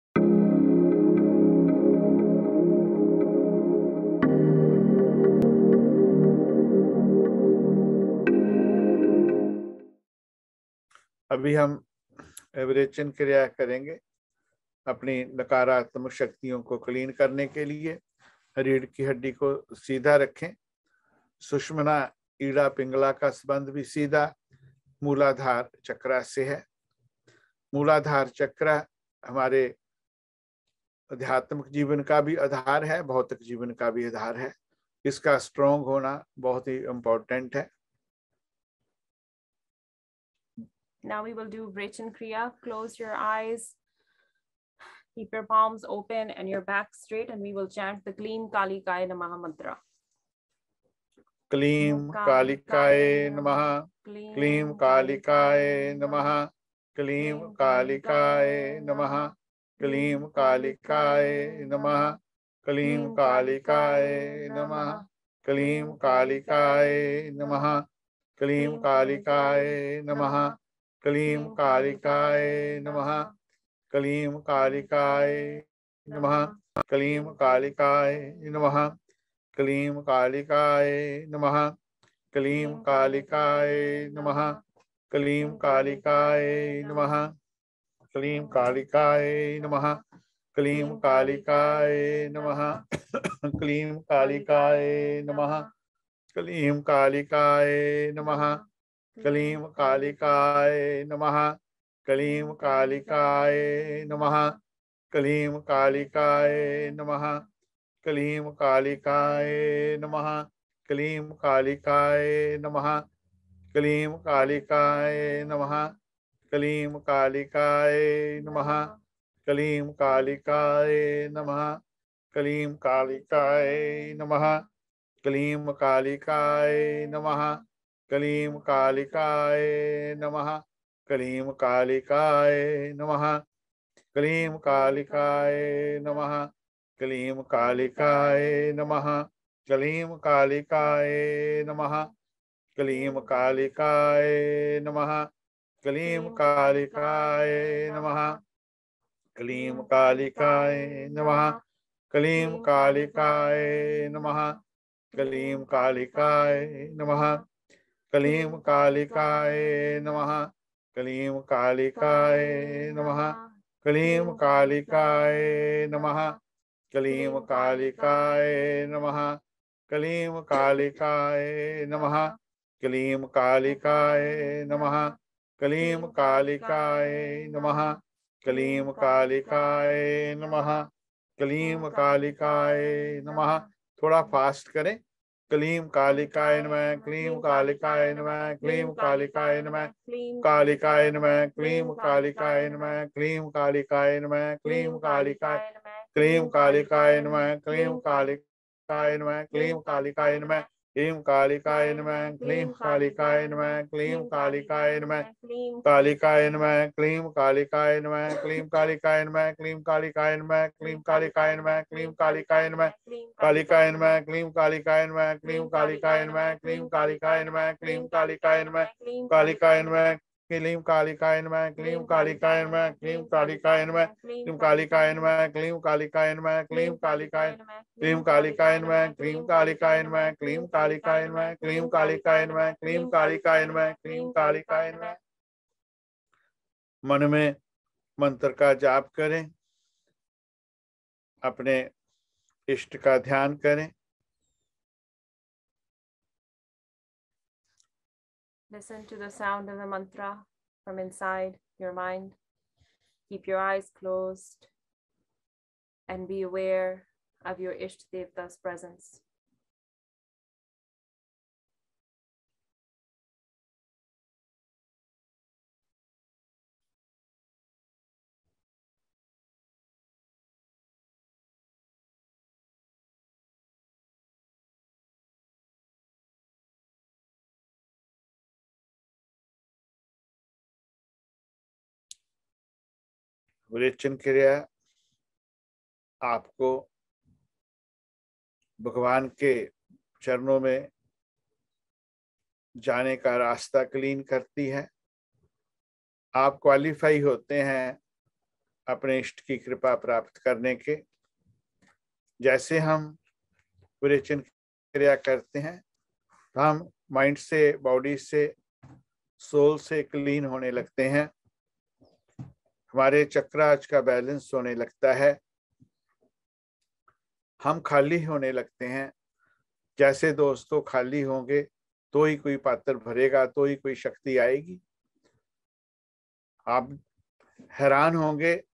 Chamunda Swami Ji
Nadi* is a channel or flow of things. The three major channels in the human body are Ida, Pingla, and Sushumna. They take care of our comfort, overall body movement, and our minds. To keep them free of any blockages and ensure smooth flow, a cleansing Sadhana is needed.

In this excerpt from the live Mantra Meditation Sadhana video, Swami Ji guides us through the Virechan Kriya for deep cleansing of all our channels.